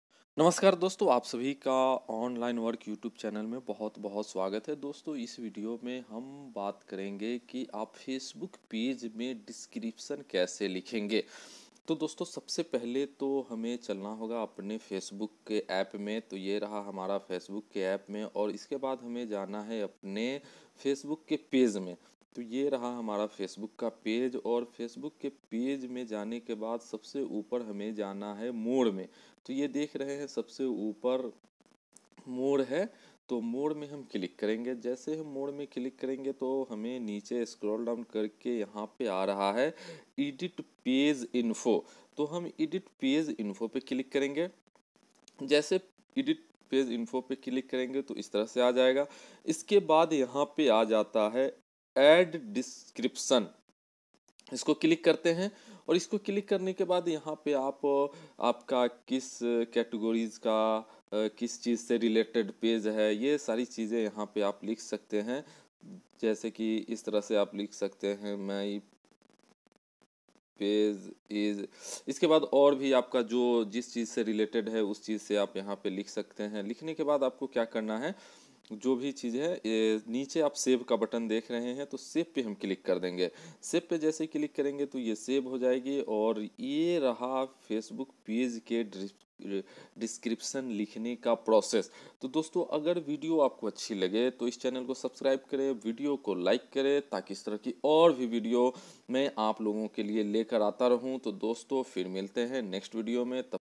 How to write description in Facebook page in hindi
Hello
Dosto is video me Mai aap ko bataya Hu ki aap Facebook page me description kaise likhe ge